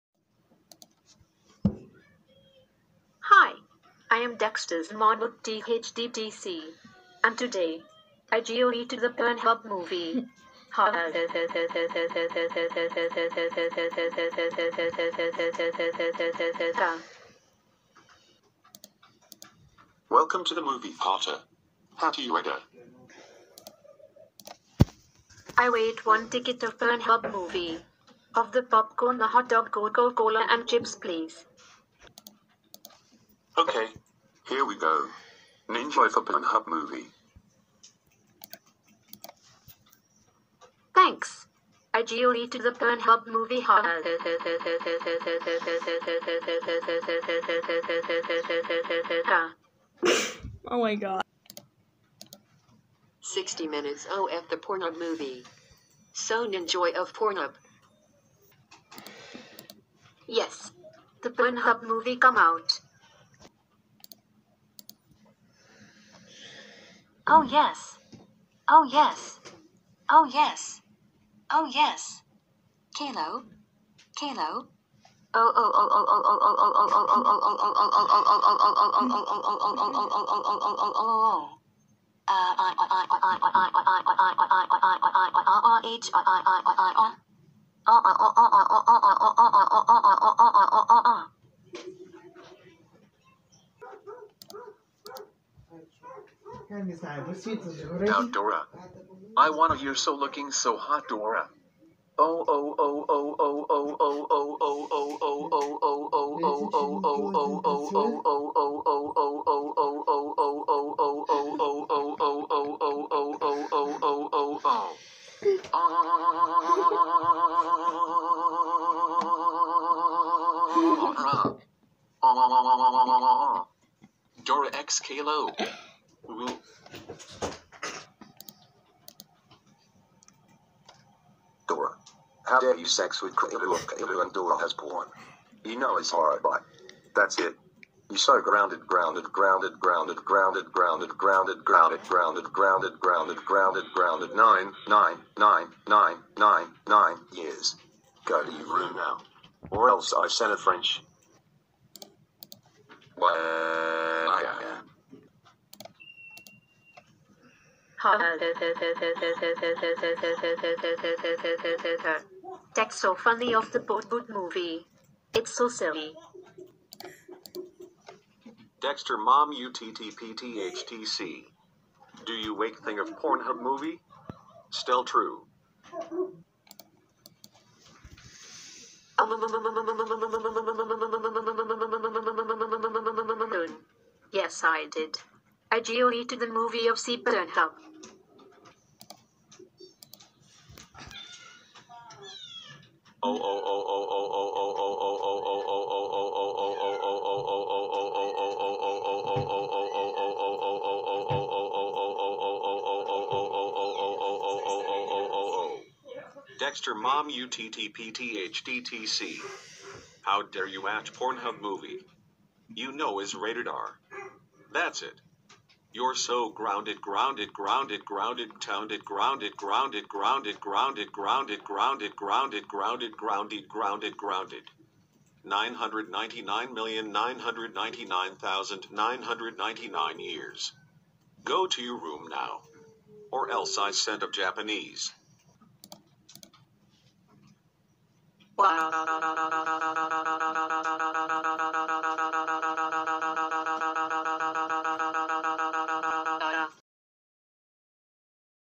Dexter's Mom UTTP THTDC Wacthing Pornhub Movie Gets Grounded
Dexter mom Uttp THDTC get grounded